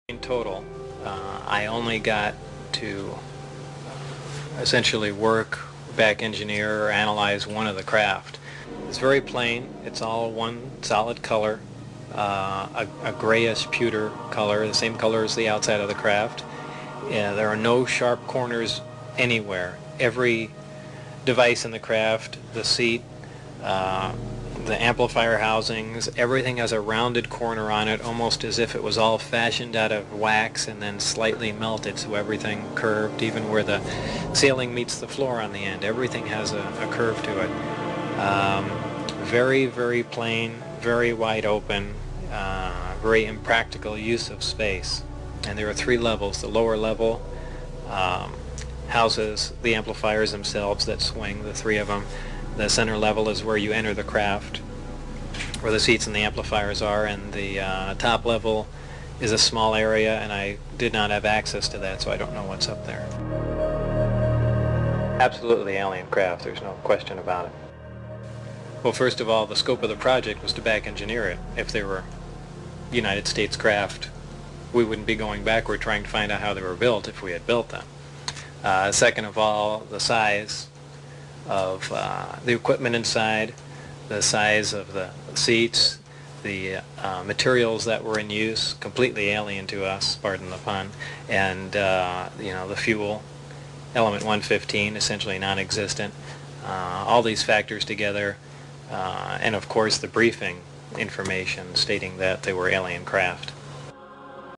UFO Lazar
Sescribes back engineering of ufo craft

8